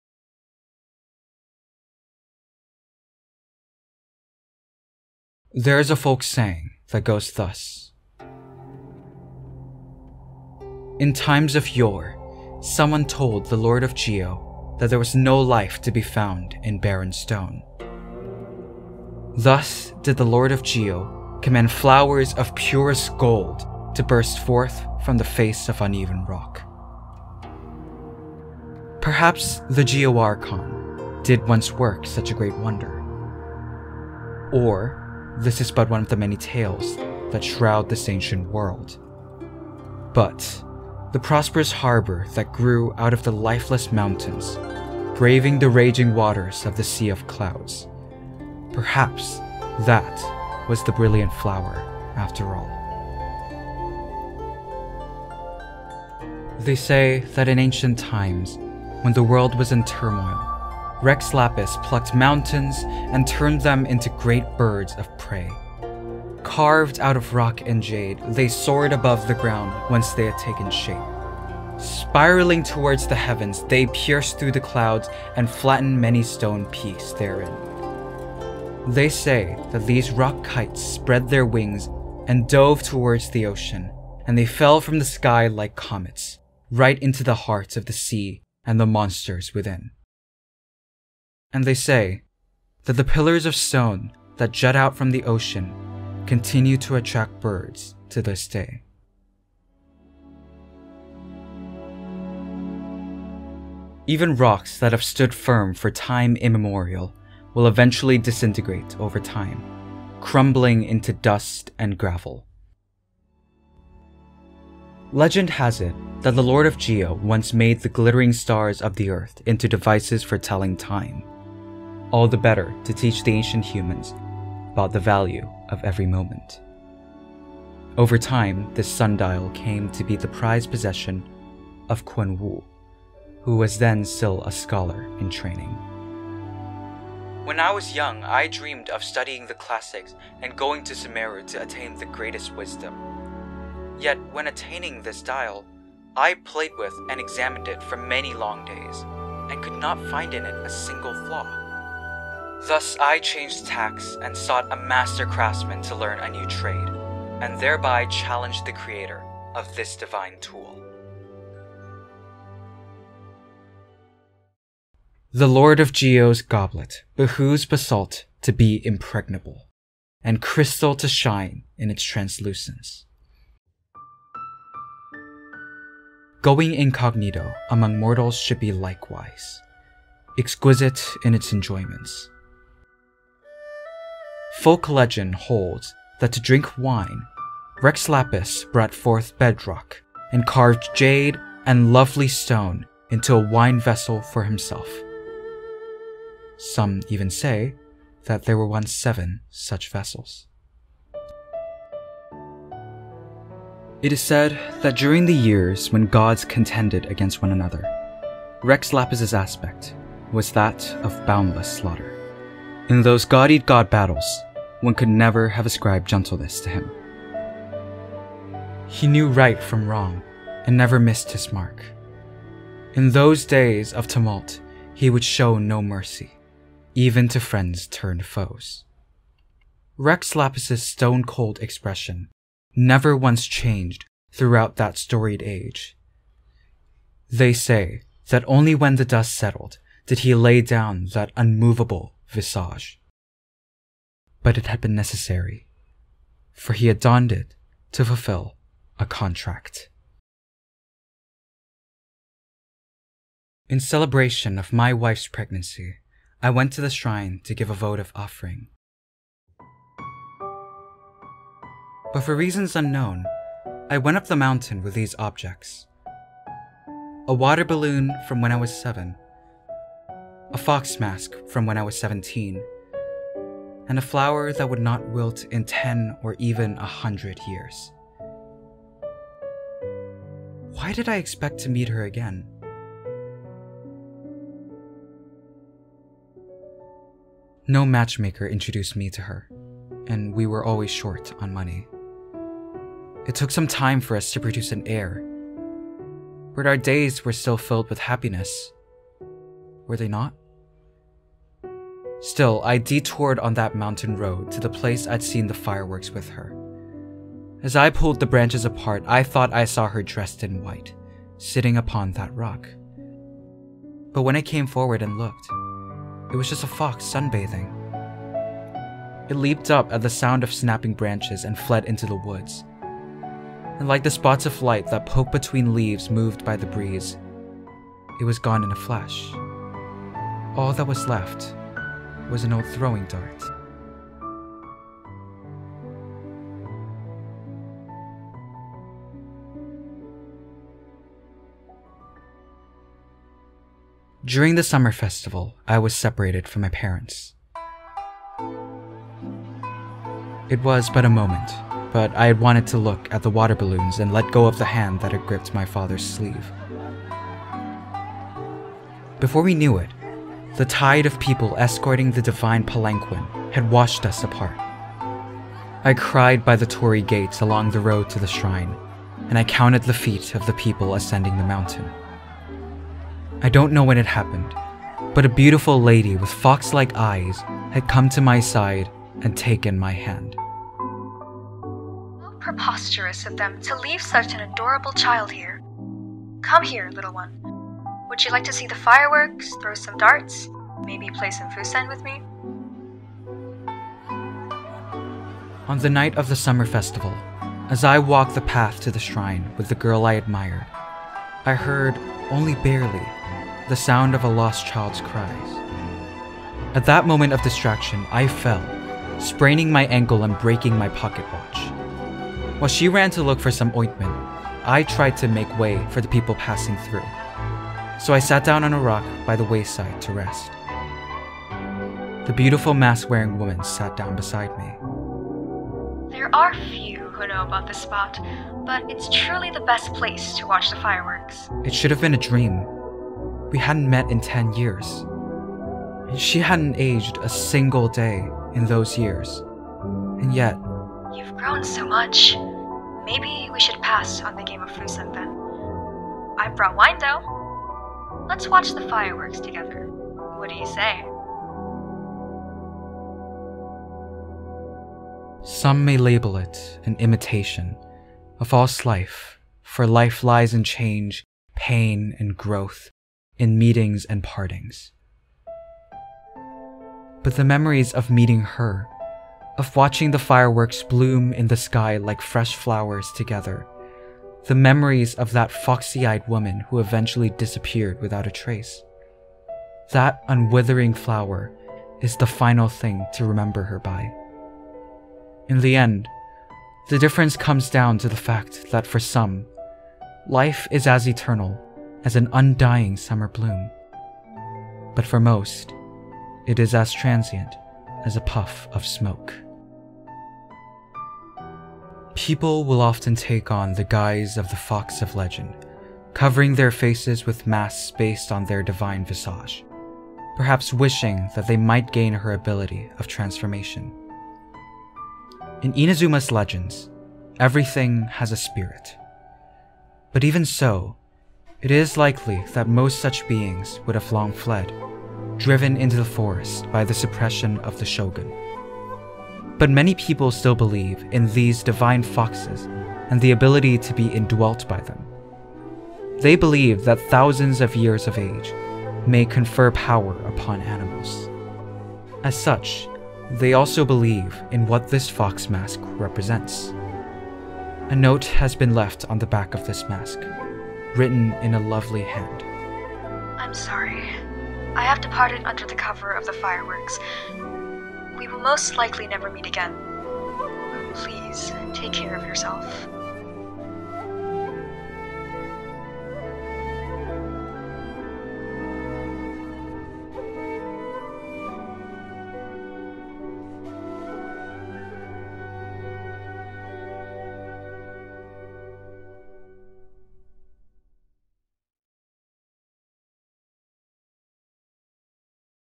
Archaic Petra & Retracing Bolide | Genshin Impact Lore Reading
There are many legends to be told about the Lord of Geo Rex Lapis, who is commonly known and revered for his feats during the Archon War, which shapes the land that he rules today. The Archaic Petra set explores his past as a warring god and his accomplishments thereafter, delving into the sacrifices he had to make for his godhood.

On the other hand, the islands of Inazuma is shrouded in mystery. One such mystery is that of Kitsune Saiguu. During the Cataclysm of 500 years ago, Kitsune Saiguu disappeared. In Retracing Bolide, we bear witness to some of her moments with a mortal, along with reflections on his mortality and her immortality.

Special thanks to my friend Vir for voicing Kitsune Saiguu, their Instagram is @questionablethespian

yeah that's it